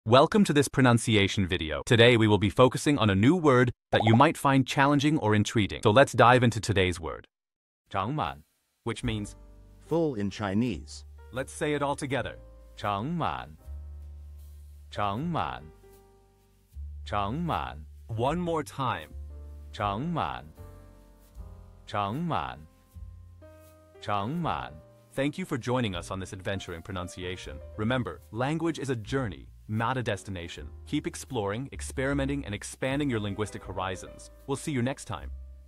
How to pronounce zhǎng mǎn | 长满 (Full in Chinese)
Master the Pronunciation of 'zhǎng mǎn | 长满' - which means : Full in Chinese 📝 with @PerfectPronunciationChinese 🎓🗣️ - [Your Guide to Chinese]

Learn to pronounce 'zhǎng mǎn' perfectly in Chinese with our easy and engaging tutorial by @PerfectPronunciationChinese! 📖📽️ In this comprehensive video, we provide you with native speaker examples 🗣️👂 and phonetic breakdowns 🎵 to ensure you get the pronunciation just right.